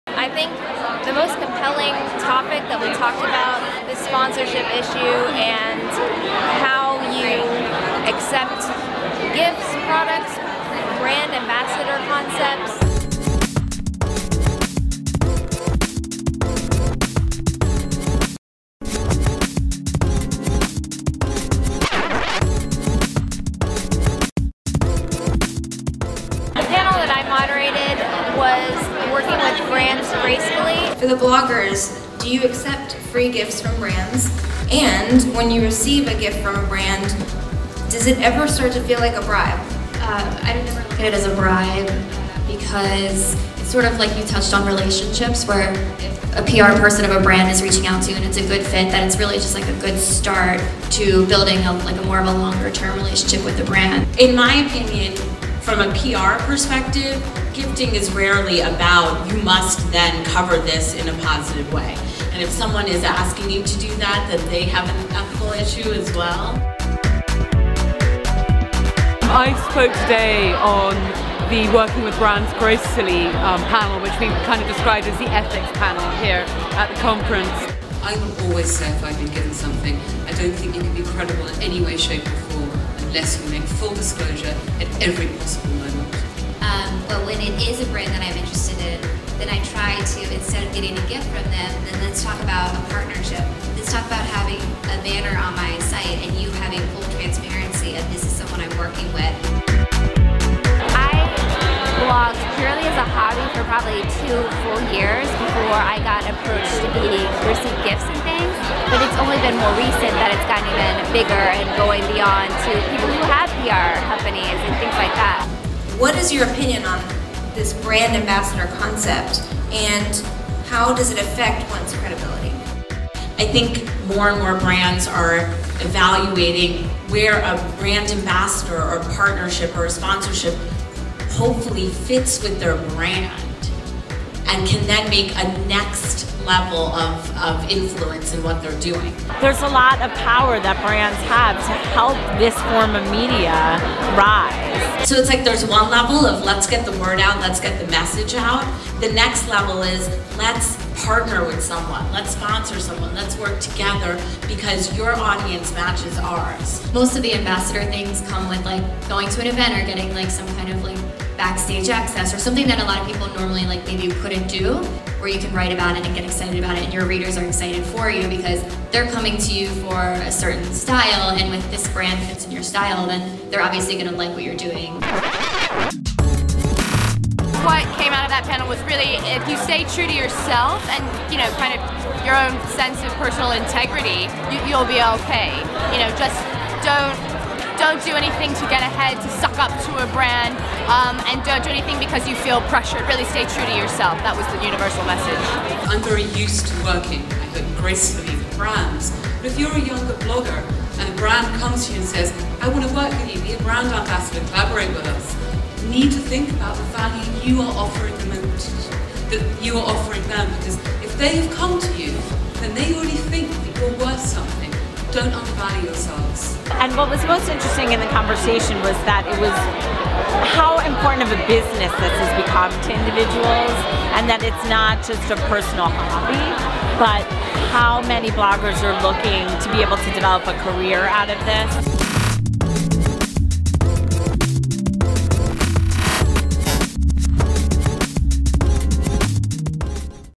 Evolving Influence: Working with brands gracefully
When you start monetizing your blog, questions start to come up. How as a single blogger can you keep the division between business and editorial?  How do you maintain your credibility with your readers, is transparency enough?

    * Defining the difference between promotional and editorial
    * Gifts or Bribes?
    * Fact checking... is Google enough?

Moderated by Kristina Medhus, Pretty Shiny Sparkly Panelists: Sasha Wilkins,  Liberty London Girl •  Rebecca Stice,  Clothes Horse • Kristin Knox, The Clothes Whisperer •  Lindsey Calla, Saucy Glossie • Rachna Shah, KCD